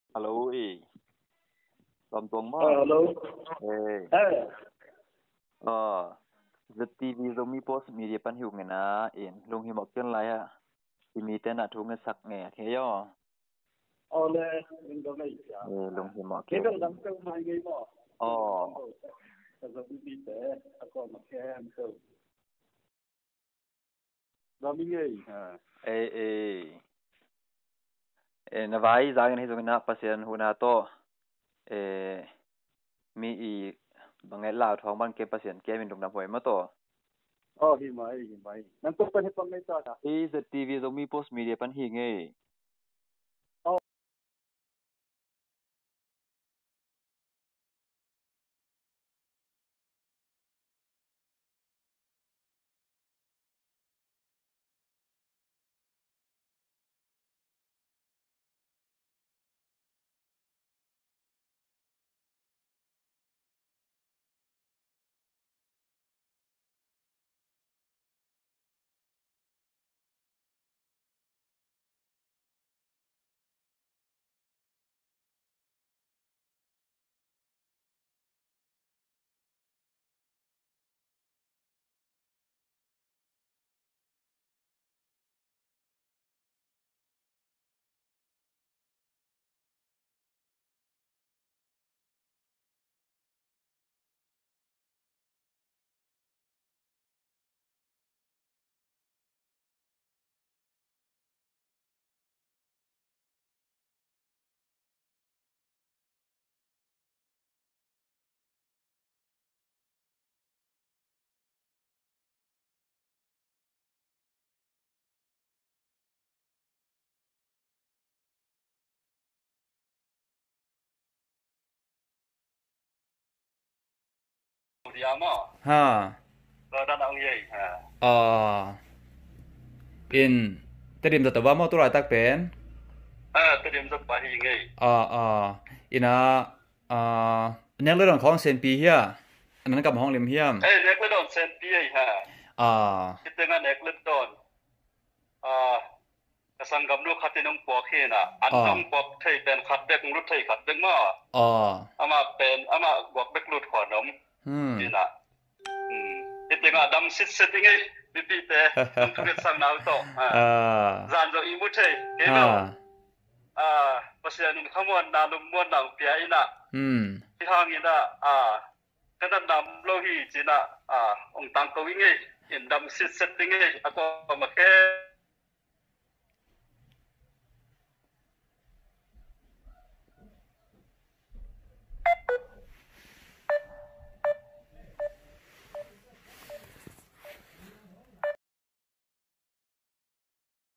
ZTV-Interview: Tedim ah Covid 19 nei kici Sanggampa tawh kihona
Tulai tak Tedim khuapi ah Covid19 damlohna hang in Syahuan te hongkepsak Sanggampa tawh kiho na hi. Pasian in hongkepsak in aciasa dammel sitset in amah zong kanatna om kei ci hi.

Zato pa hongtuakbaih na ding thungetsak ni